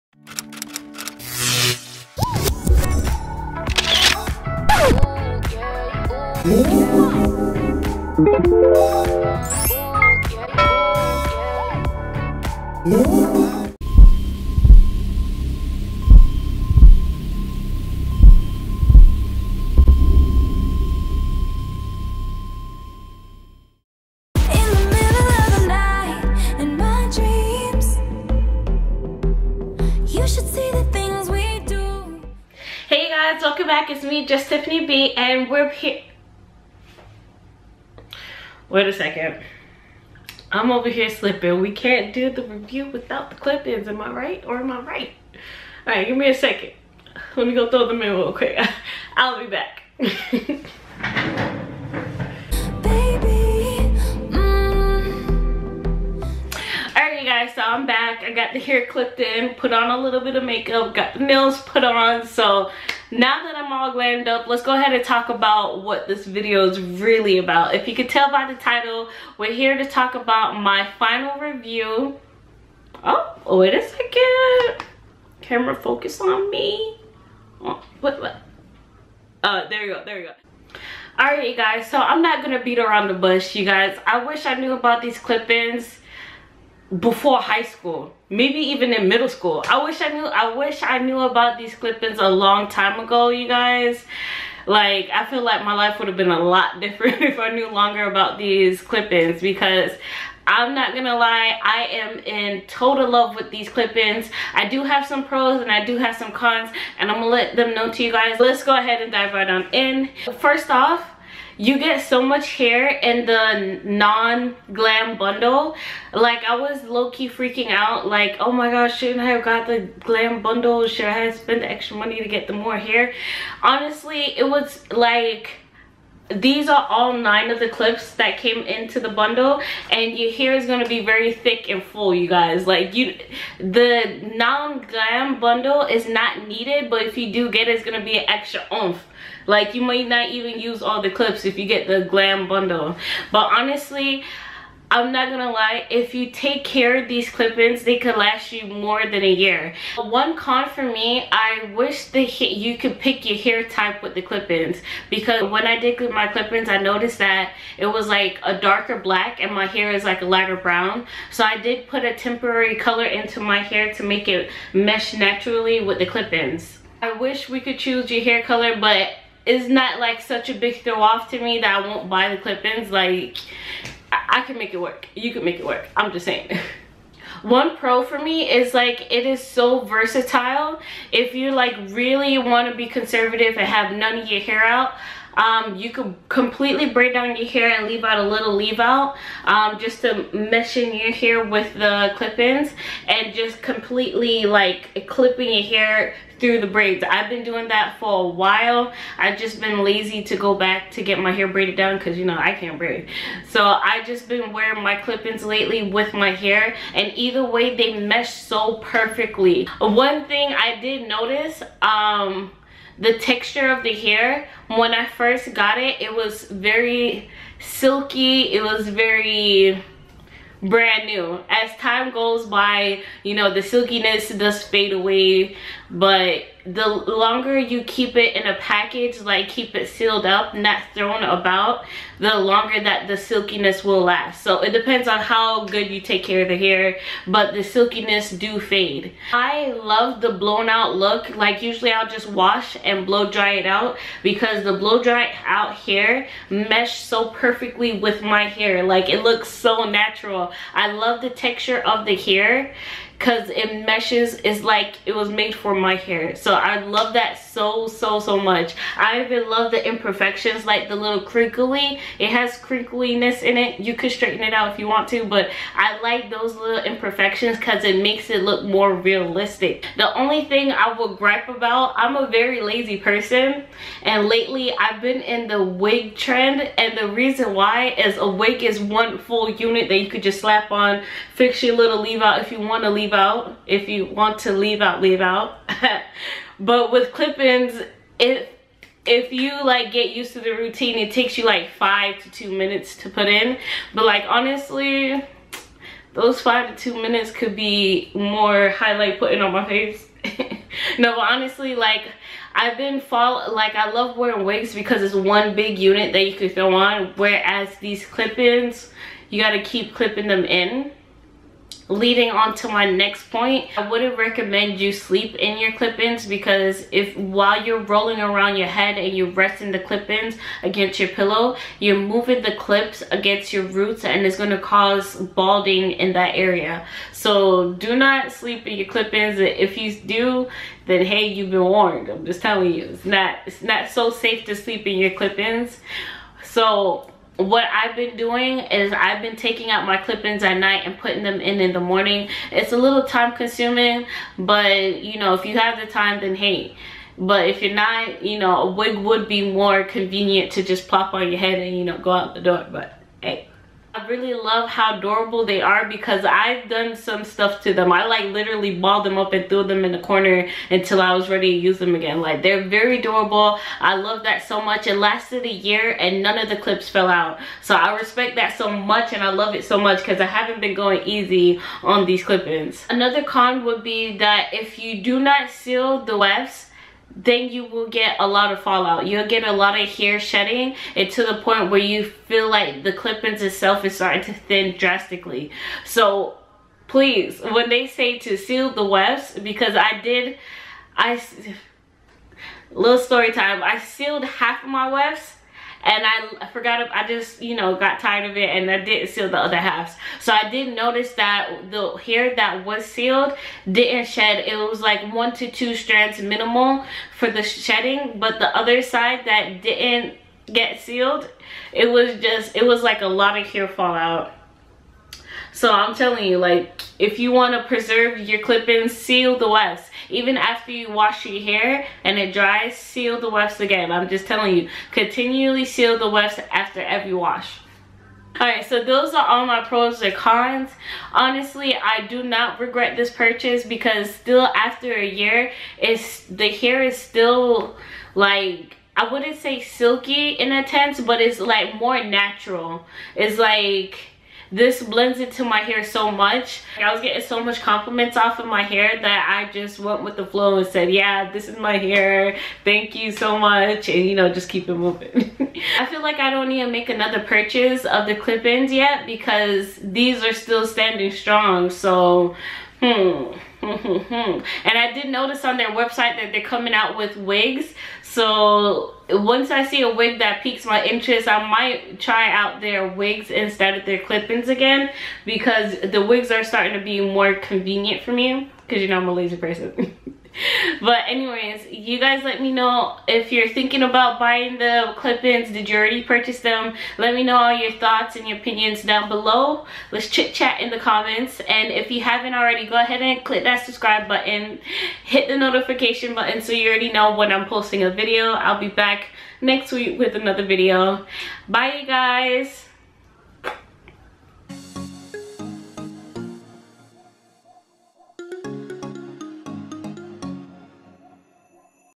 Knappy Hair Extensions Not So Knappy Clip Ins | One Year Review
Hey guys, Im wearing the not so knappy hair extension clip ins! The length is 18" not the glam set!

♥ I N S T A G R A M: @justtiffanyb